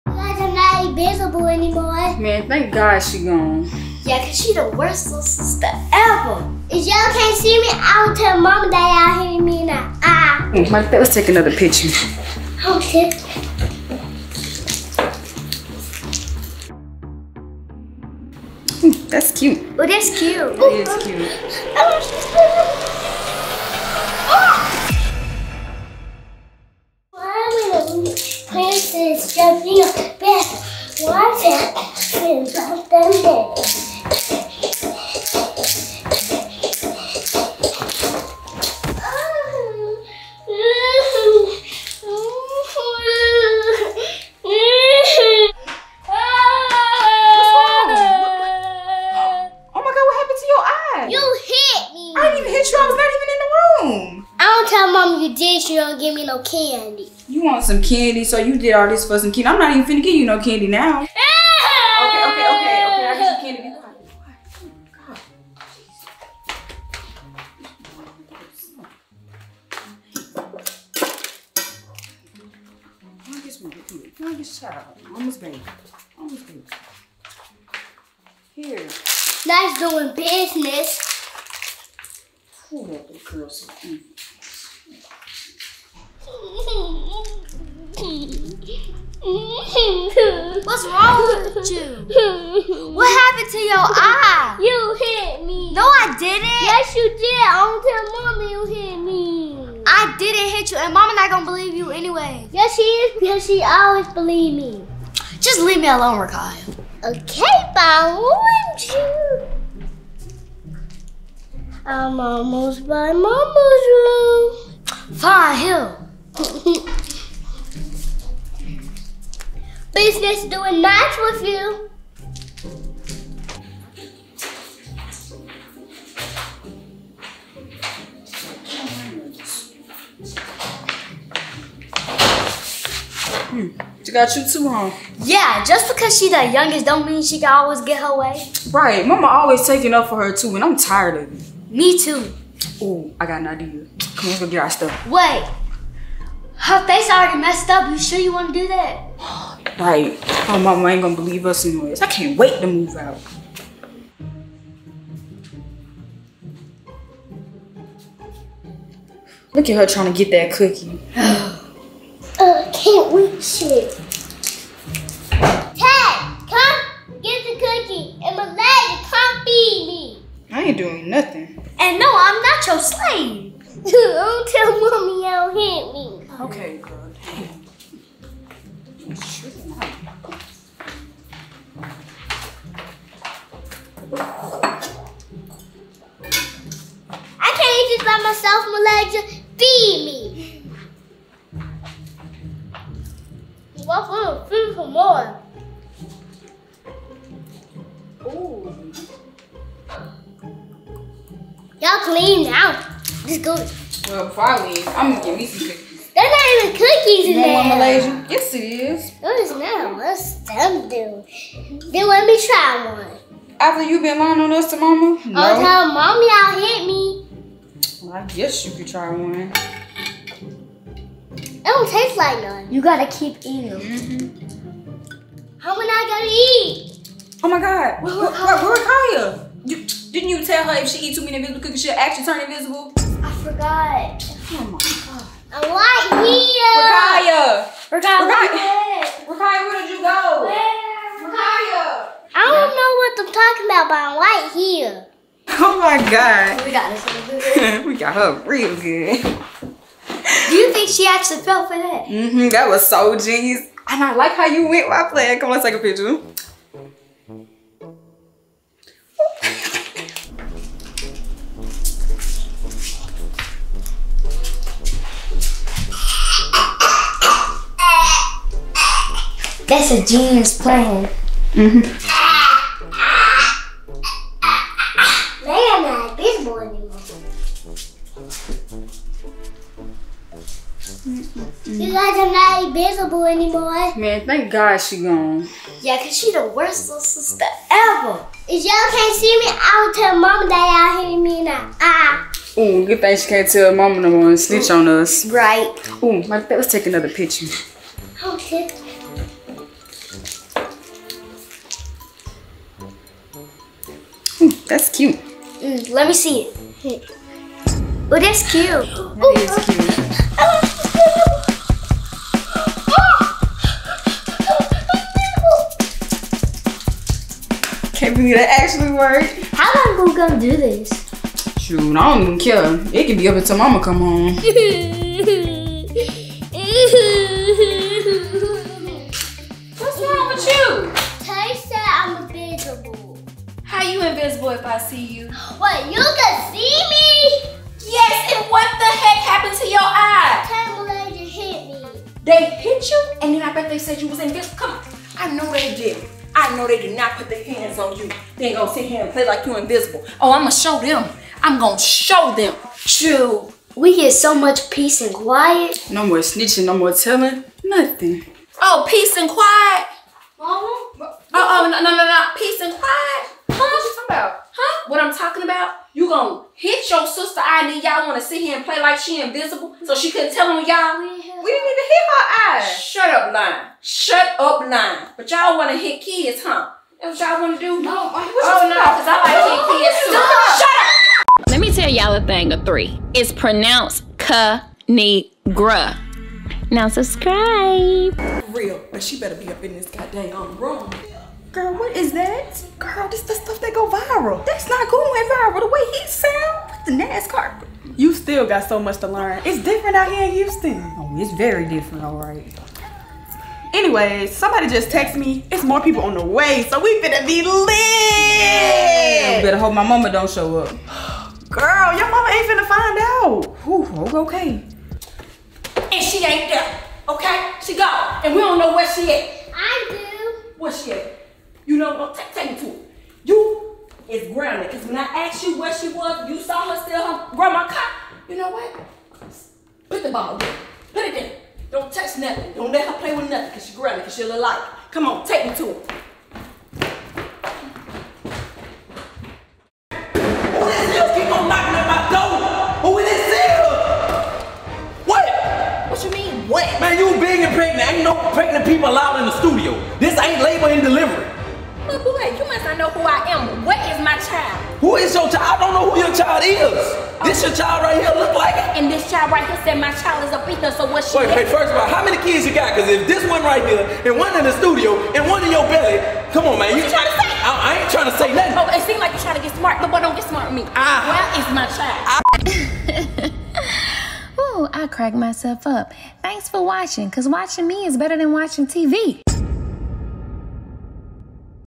THE YOUNGEST CHILD 👧🏾❌| Ra’Kiyah gets a black eye 🤜🏾🤕 (Season Finale)
THE YOUNGEST CHILD| INVISIBLE PRANK ON RAKIYAH
The older siblings decide to get pay back on their youngest sibling by telling her she is invisible.

Created by: @KinigraDeon
Starring: @KinigraDeon @MALAYSIARAKIYAH That Girl Tae @KinigraDeonFamily
Written by: Keisha & Dame Pierre

Be Sure to Follow all my other platforms and channels ❤️